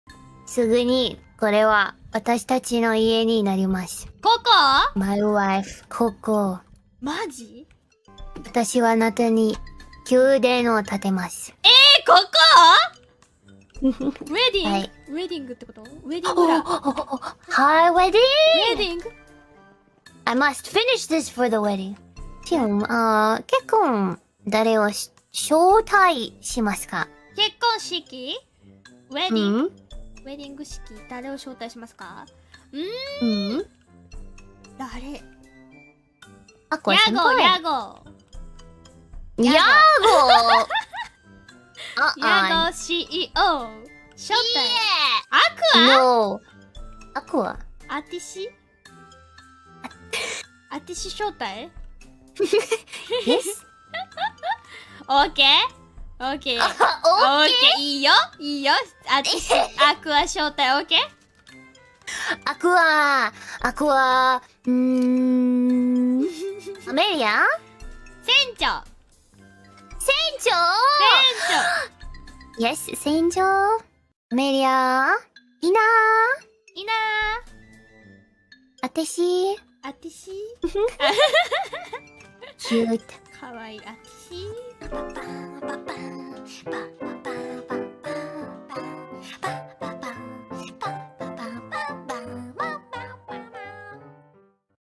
Gura y Shion invitarán a sus Novias a la BODA (Sub Esp)
¿Pensaste que era solo Homolive? No, tambien es NTRLive.

Si tienes alguna sugerencia de algún clip que quieras ver traducido hazmelo saber en los comentarios.

Puedes apoyarme con una pequeña donación :)